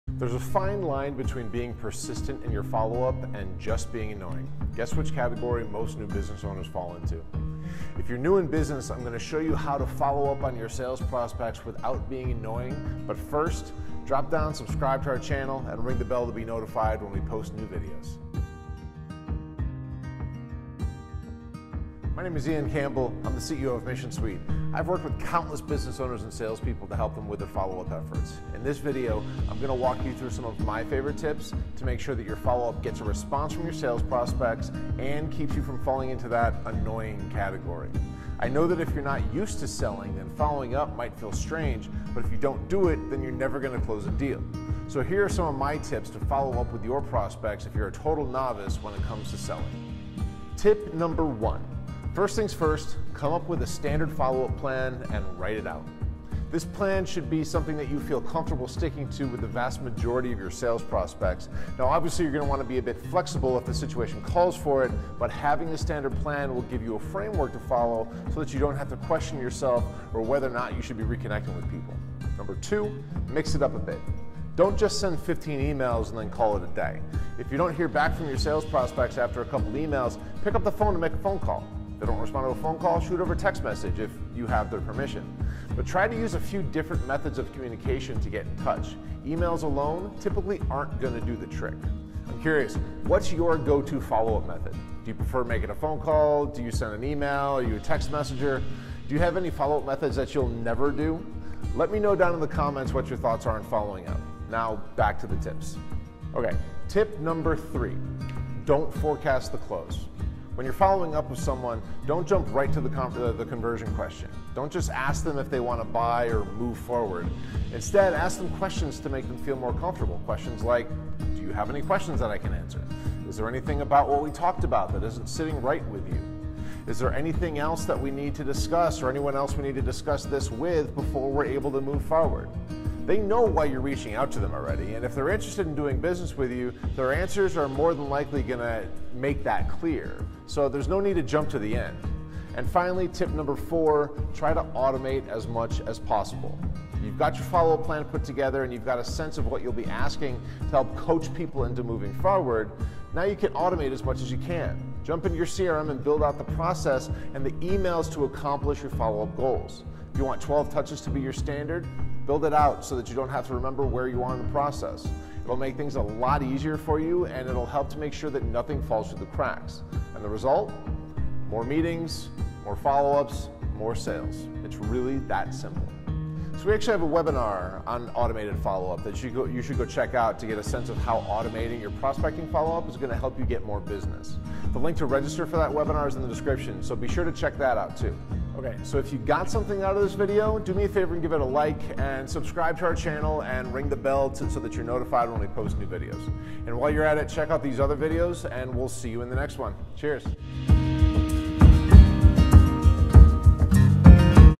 How to Follow Up With Sales Prospects for Beginners
Learn how to follow up with prospects to boost your sales and open opportunities with these follow up tips and strategies. This video will help you with closing your sales funnel and prospect without being annoying. This sales funnel secret can help you grow your business and increase your sales. You can learn how to make an effective sales follow up strategy to give the illusion that you have a lot of experience in sales!